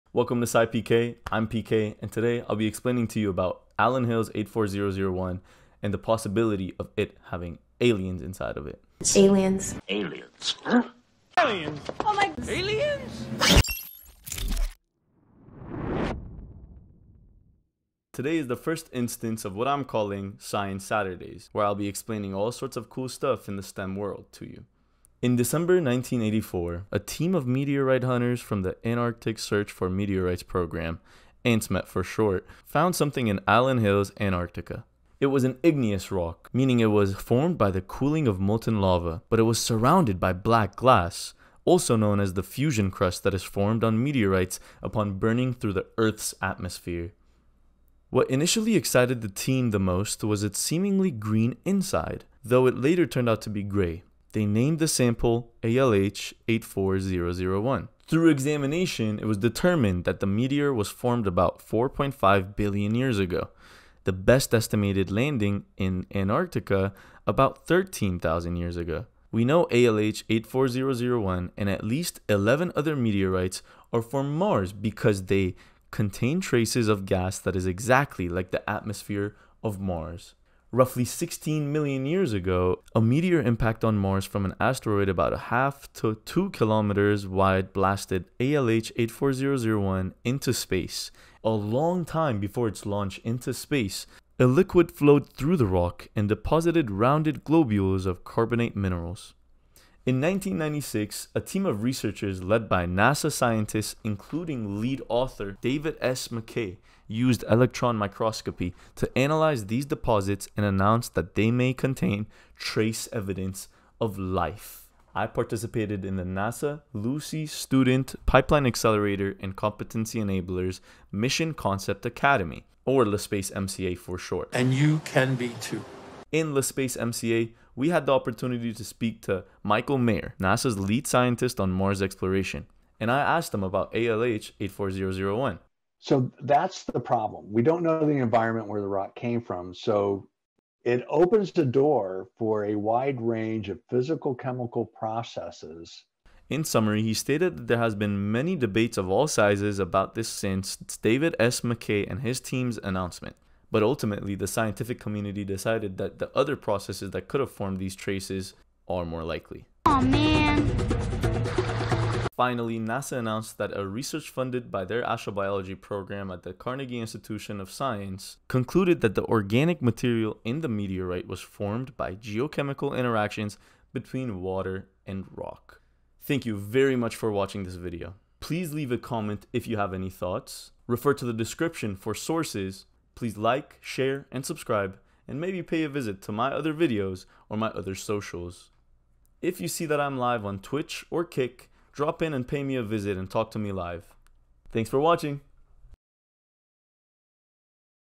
MARTIAN ALIENS FOUND IN ANTARCTICA OR A HOAX? (the story of ALH 84001)
Do aliens exist are aliens real and do martians exist. A Martian Meteorite found in Antarctica has worm-like potential alien microfossils inside of it. Could this be proof of a more lively history for Mars or is this just a natural chain structure that resembles a microfossil.

In the first episode of Science Saturdays, learn about the first MARTIAN METEORITE discovered; and the controversy surrounding the discovery of ALIEN microfossil-resembling chain structures within it.

Allow me to tell you about the creation of this igneous rock, its launch into space from Mars, its discovery in Allen Hills, Antarctica, and the scientific debate around an article published by NASA's pioneering Astrobiologist, David S. McKay.

Sources:
Many pictures from NASA were used.
“What Is ALH 84001?” Lunar and Planetary Institute,
Scalice, Daniella. “An Update from ALH84001.” NASA, 18 Jan. 2022, astrobiology.nasa.gov/news/an-update-from-alh84001/.
McKay, David S., et al. “Search for Past Life on Mars: Possible Relic Biogenic ...” Science.Org, 16 Aug. 1996,

Socials:
Gaming Channel - @SciPKUnprofessional